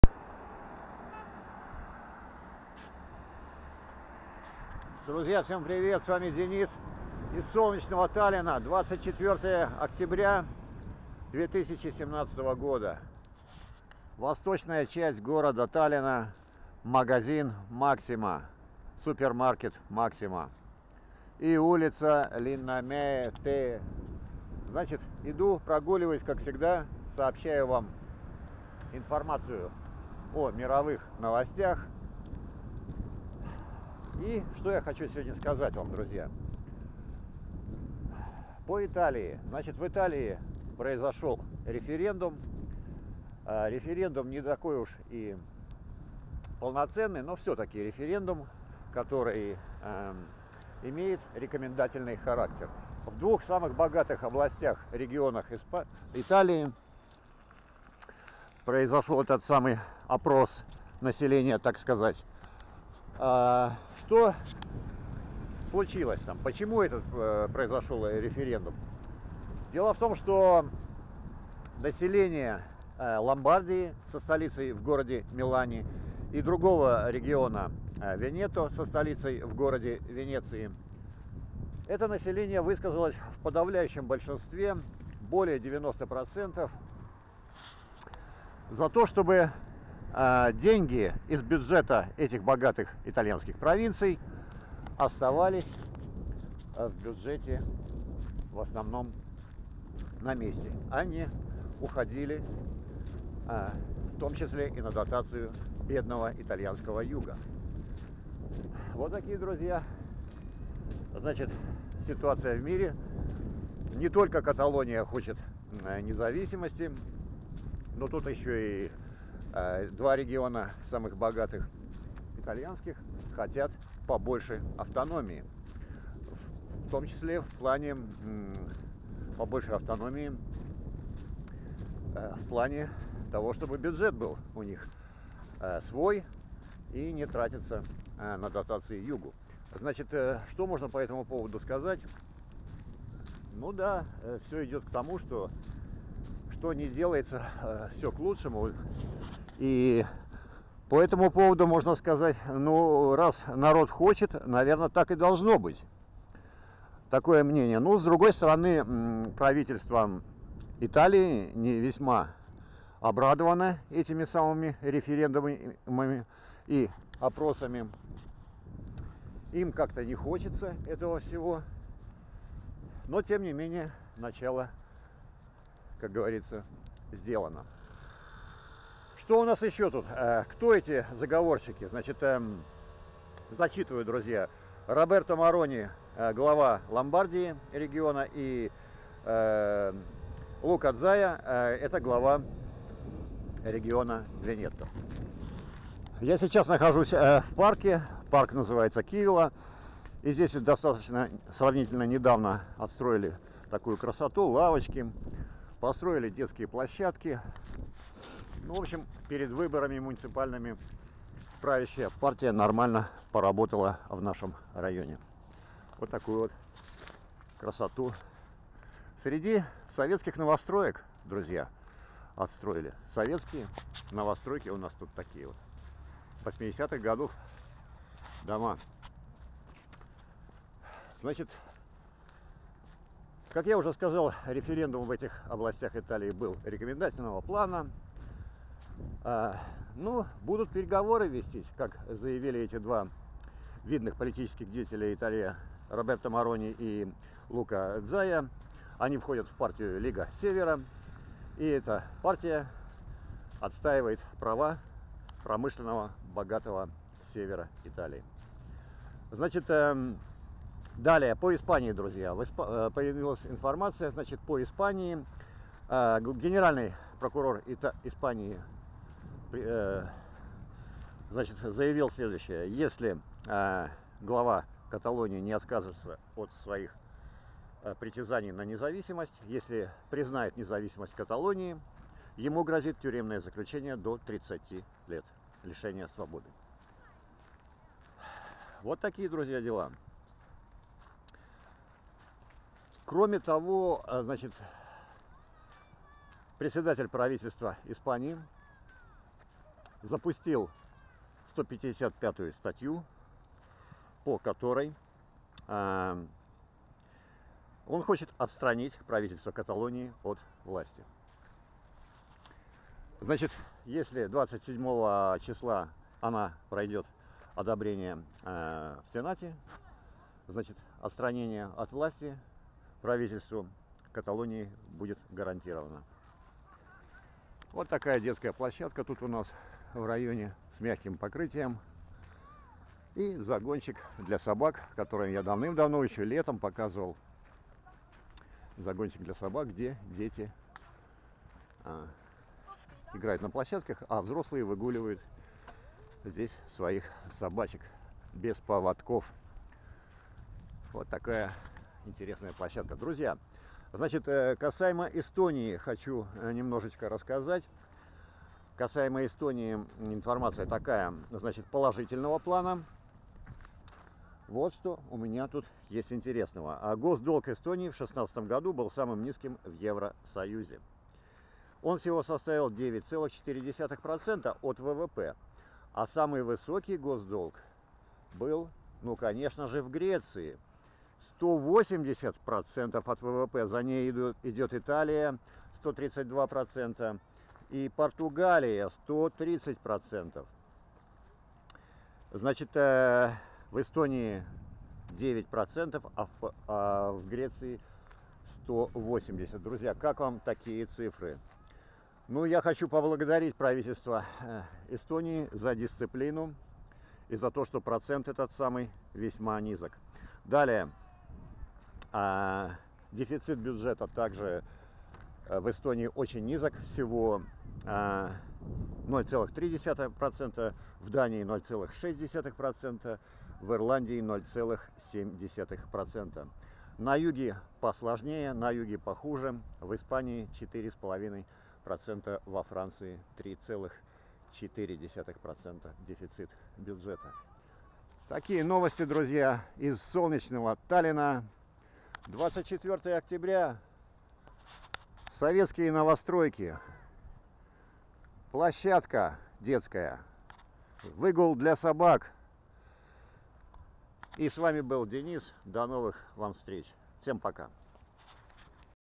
ЭСТОНИЯ/ИТАЛИЯ/ИСПАНИЯ
ГОСДОЛГ ЭСТОНИИ В 2016 ГОДУ СОСТАВИЛ 9,4% ОТ ВВП. РЕКОРДСМЕНЫ ГРЕЦИЯ 180%,ИТАЛИЯ 132%, ПОРТУГАЛИЯ 130% ГОСДОЛГА ОТ ВВП/РАЙОН ЛАСНАМЯЭ/УЛИЦА ЛИННАМЯЭ